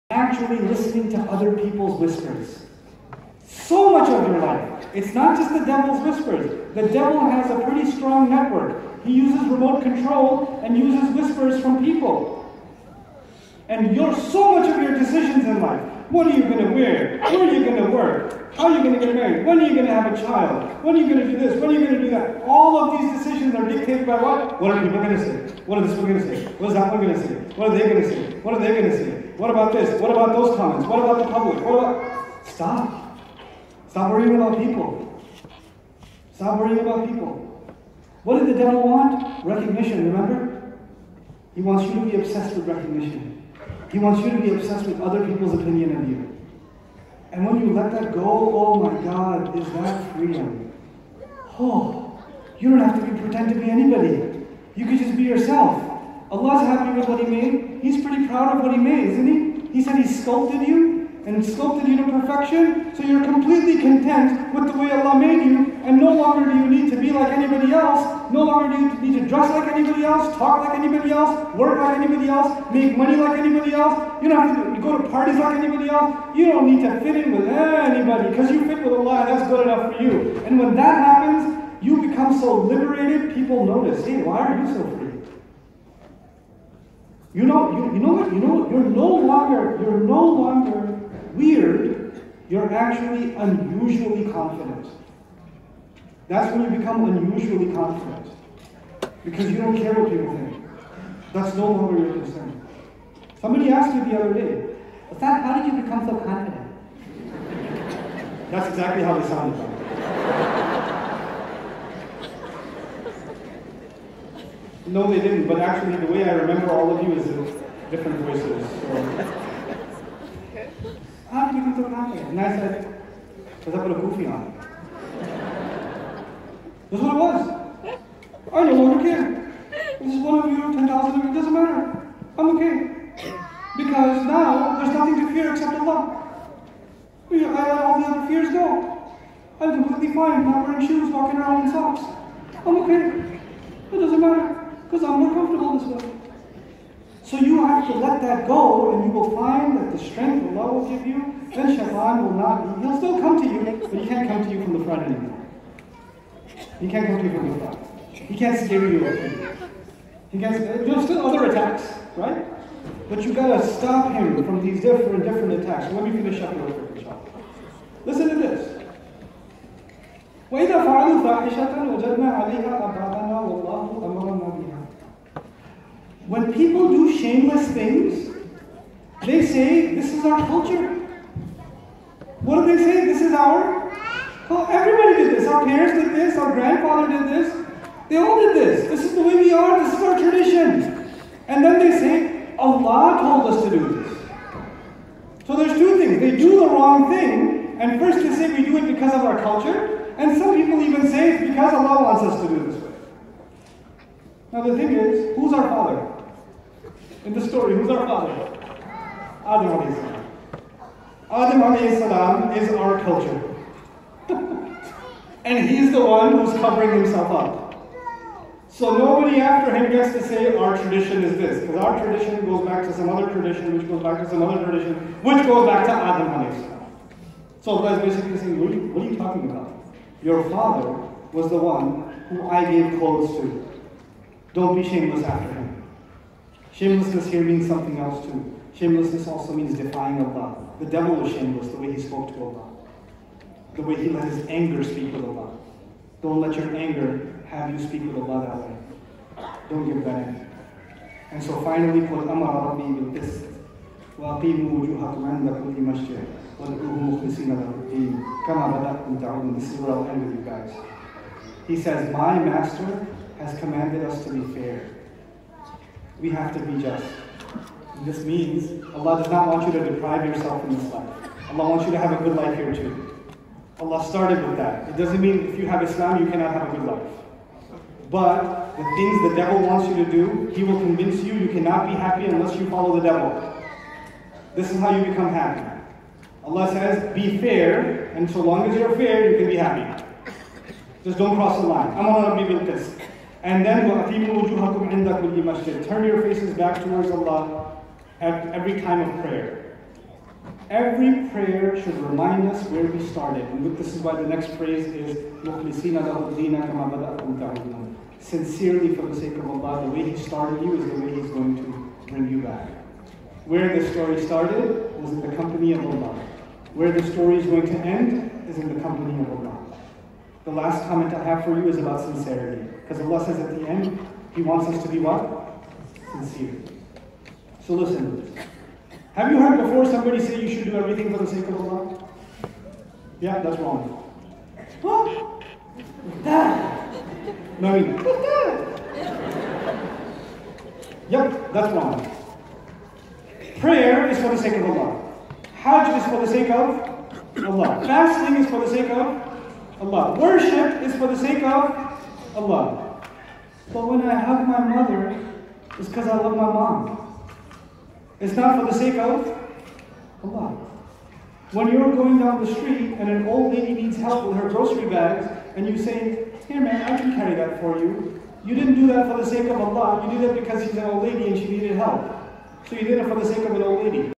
Listening To Other Peoples Whisphers
Excerpted from Noaman Ali Khan's talk titled Story Nights held on 20 July 2019 in Kuala Lumpur. The story focused on the creation of Adam, the Satan's disobedience and the way in which he made Adam slip. This is the story of you and me, in the speaker's words. This clip is about how to gain confidence and avoid falling to satan's whispering